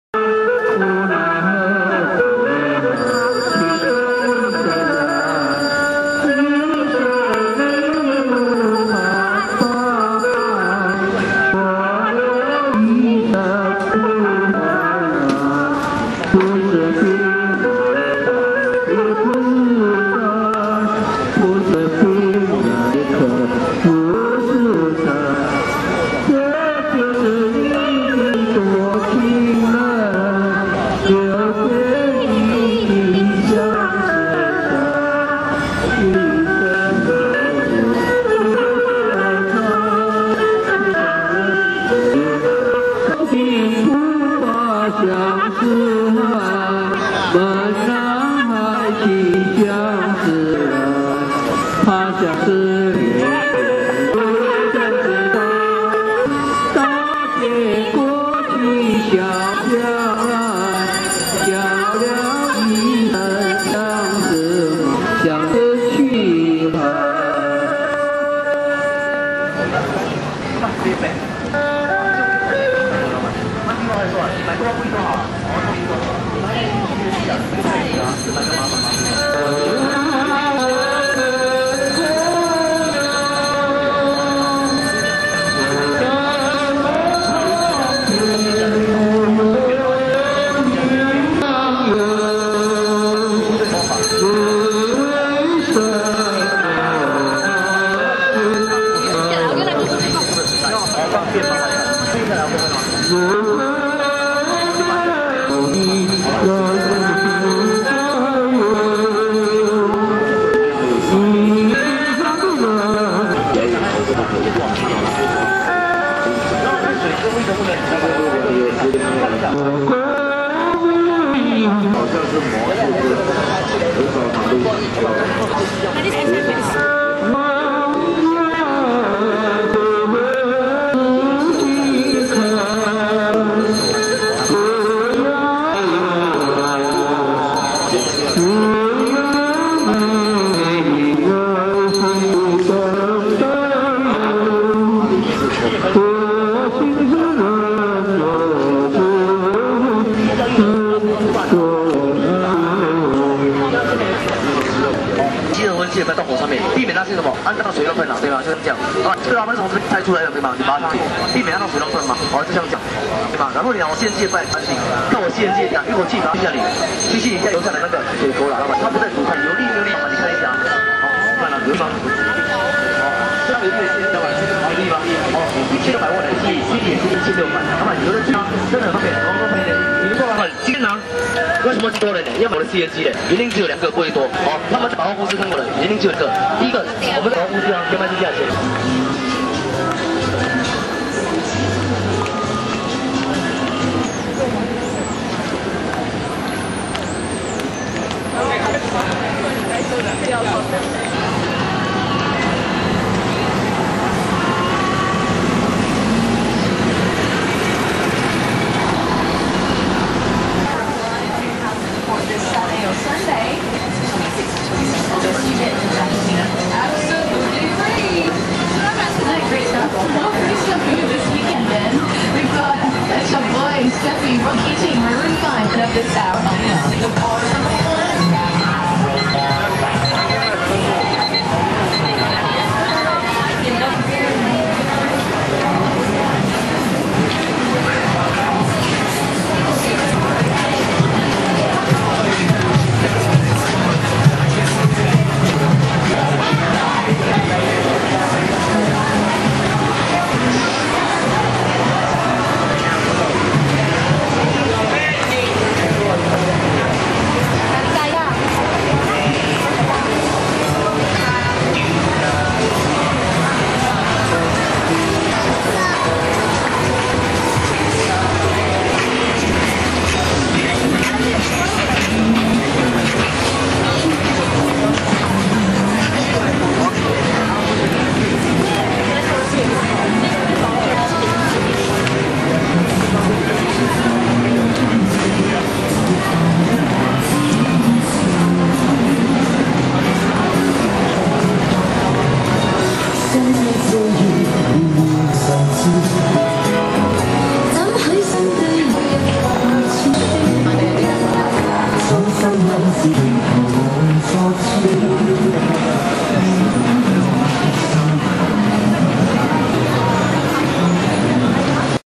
Busking - Apek Style
Saw these old folks singing along @ Daimaru,Bugis. I thought it was interesting so I took this video